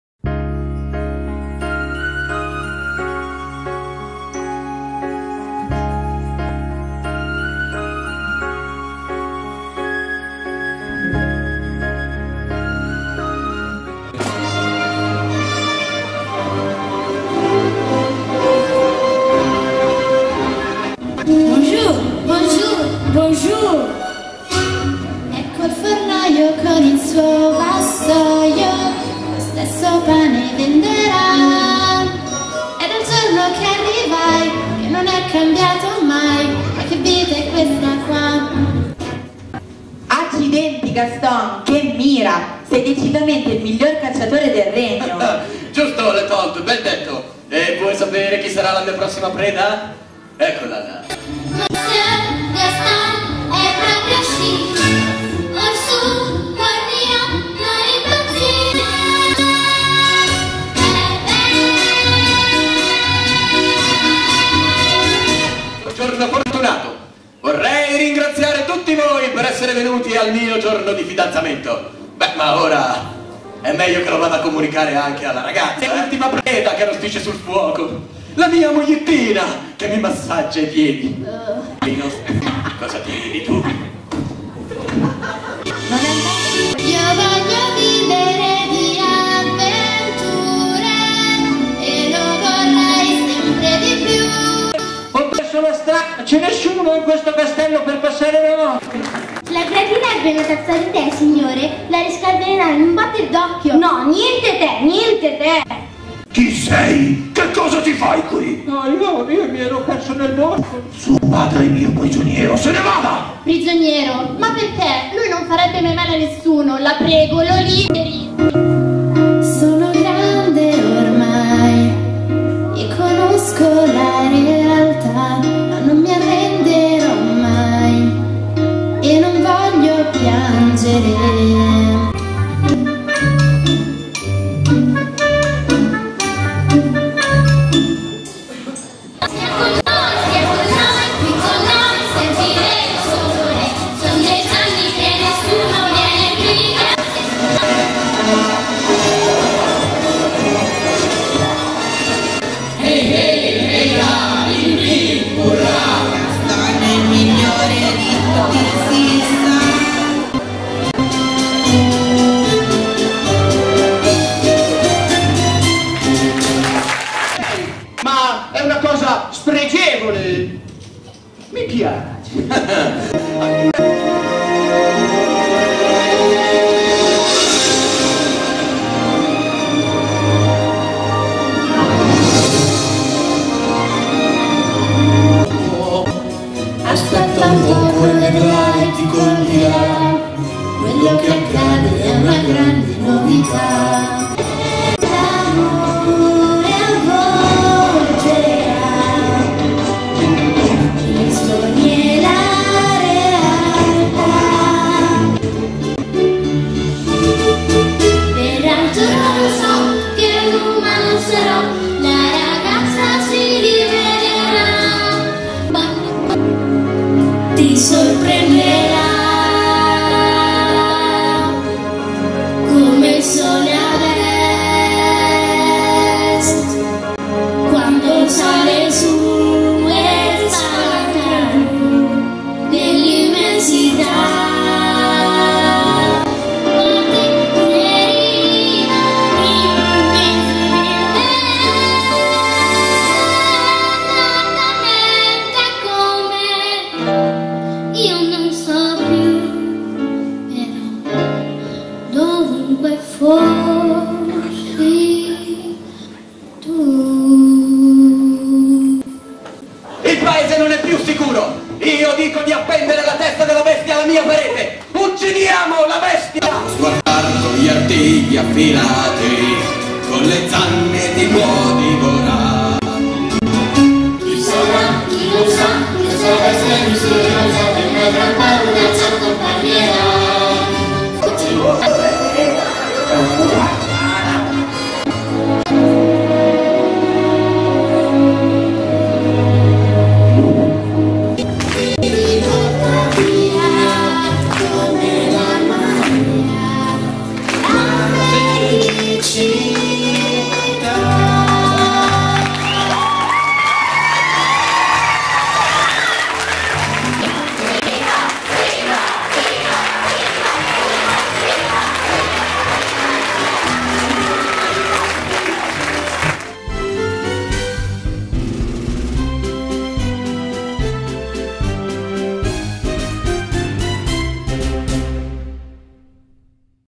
L'Amore di Belle - Trailer
Compagnia Teatrale "La Fabbrica dei Sogni" di Castelletto di Senago presenta: "L'Amore di Belle", musical tratto da "La Bella e la Bestia"